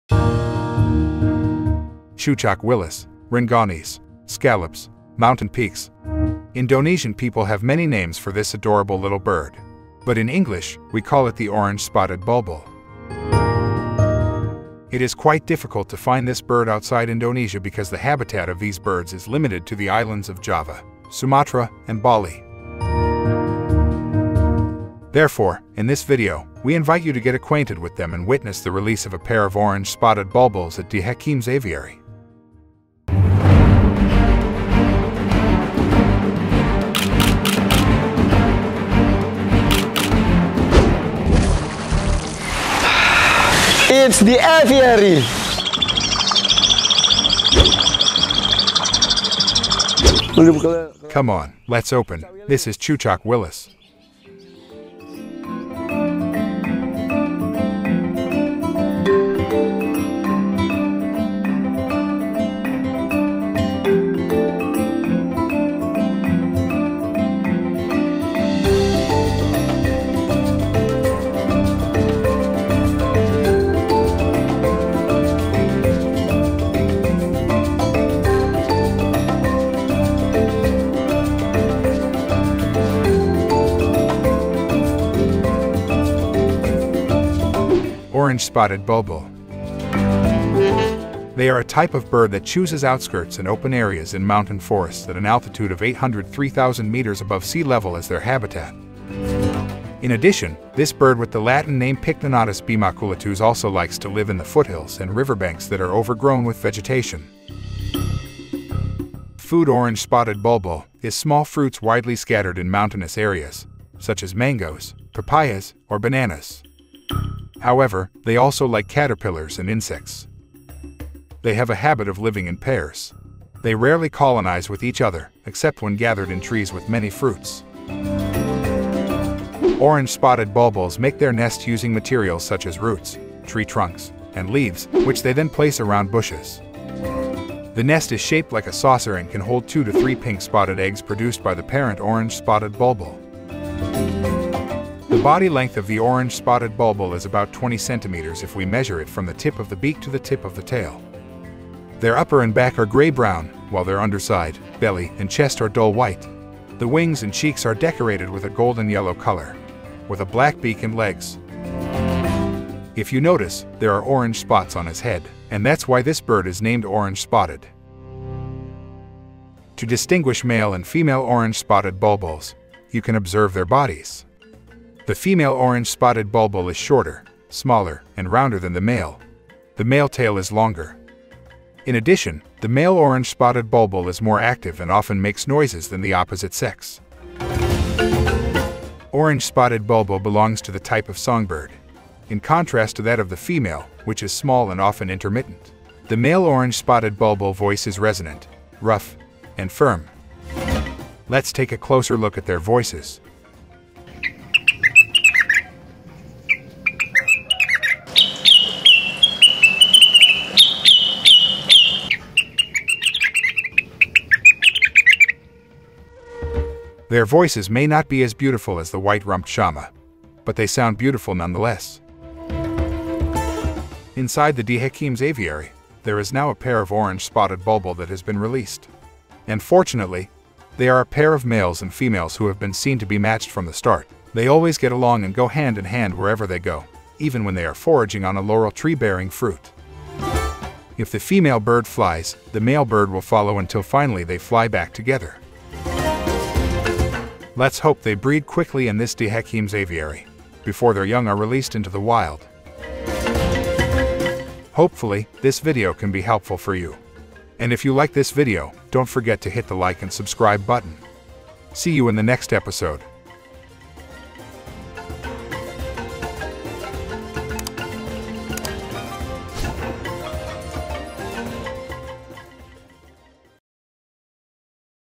Meet The Orange-Spotted Bulbul, The Cute Mountain Birds - 7th Episode
Meet The Orange-Spotted Bulbul, The Cute Mountain Birds

Cucak Willis, Rengganis, Scallops, Mountain Peaks. Indonesian people have many names for this adorable little bird. But in English, we call it the Orange-Spotted Bulbul. It is quite difficult to find this bird outside Indonesia because the habitat of these birds is limited to the islands of Java, Sumatra and Bali. Therefore, in this video, we invite you to get acquainted with them and witness the release of a pair of Orange-Spotted Bulbuls at deHakims Aviary.

For more about The Orange-Spotted Bulbul, let's watch this episode.